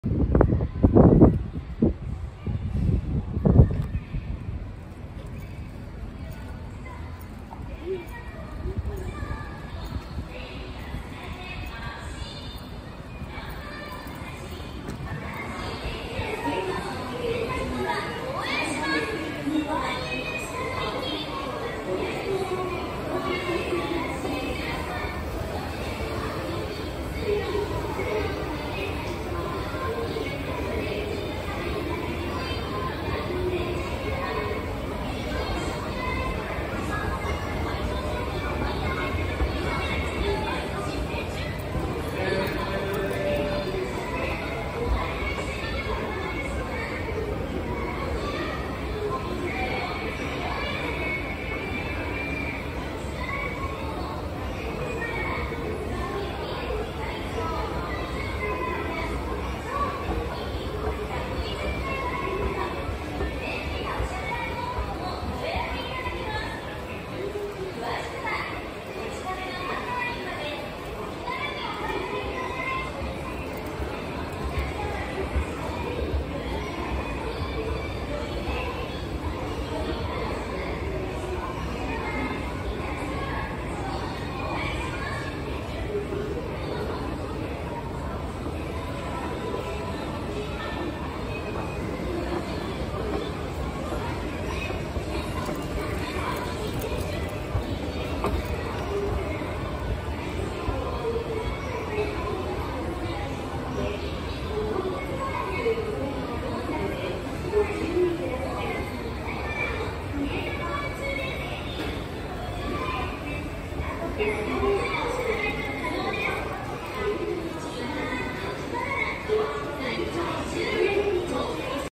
コジマ×ビックカメラ新潟店 エスカレーター escalator Kojima × Bic camera Niigata Japan
バイパスの近くにあります。エスカレーターがあってよかったです🤭